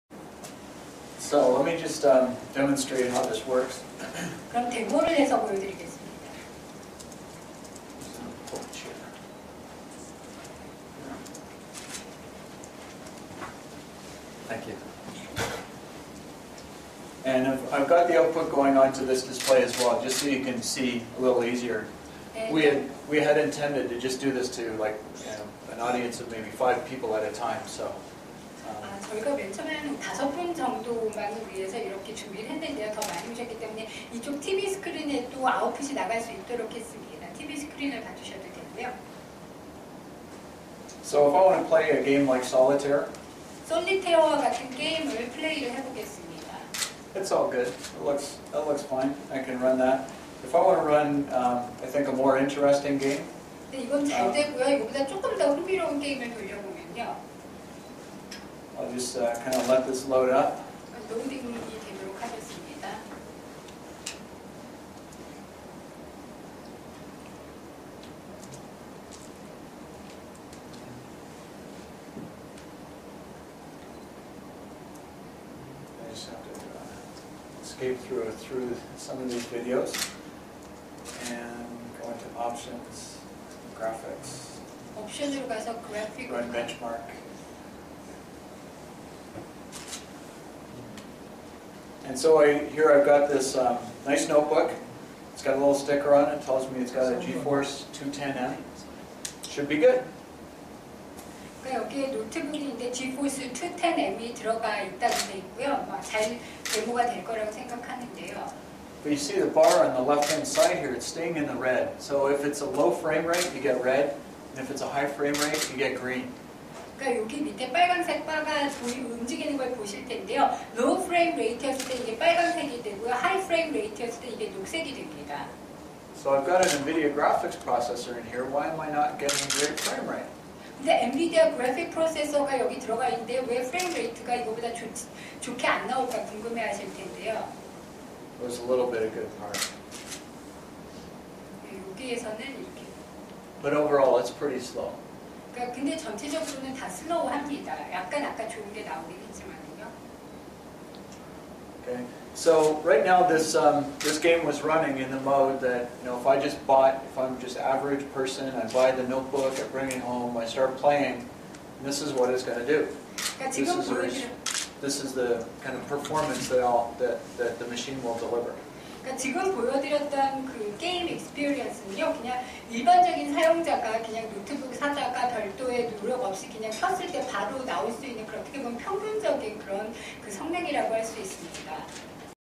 NVIDIA Optimus Technology DEMO : Part 1/2
NVIDIA Optimus Technology DEMO - Intel GMA Graphics based on ASUS UL50Vt Notebook PC (Record : Acrofan)